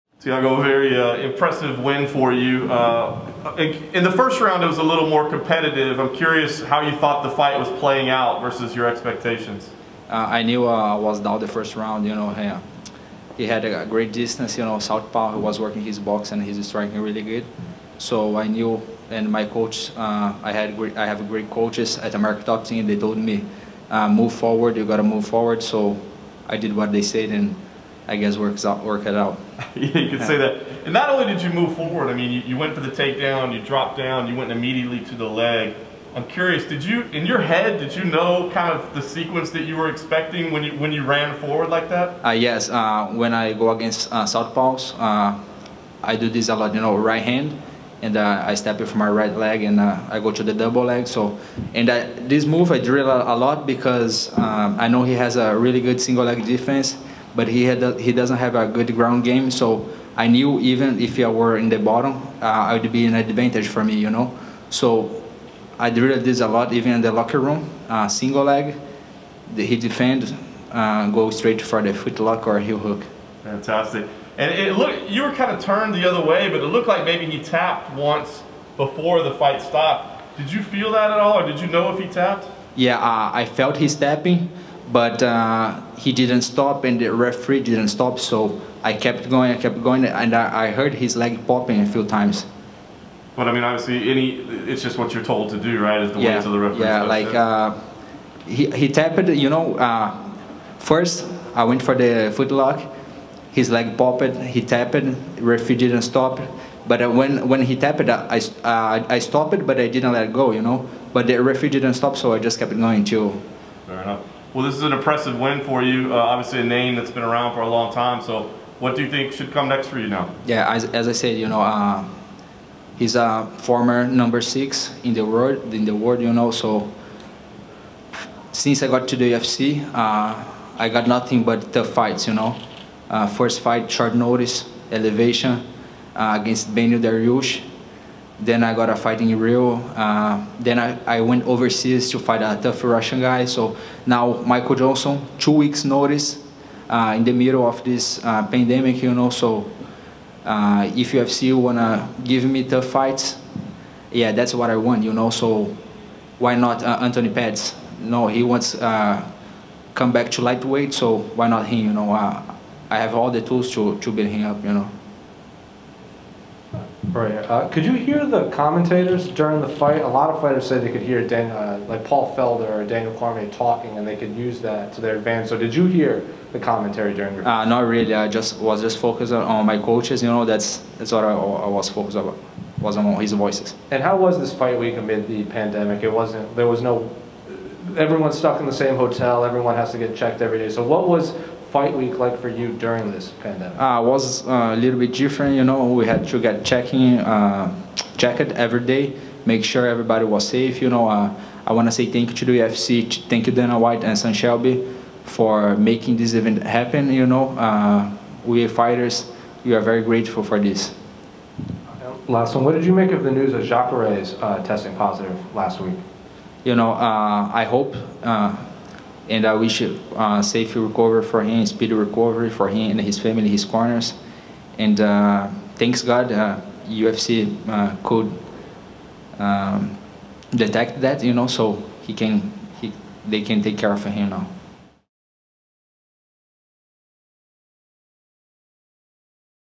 UFC on ESPN+ 29: Thiago Moises full post-fight interview
Thiago Moises spoke to the media following his win at UFC on ESPN+ 29 in Jacksonville, Fla.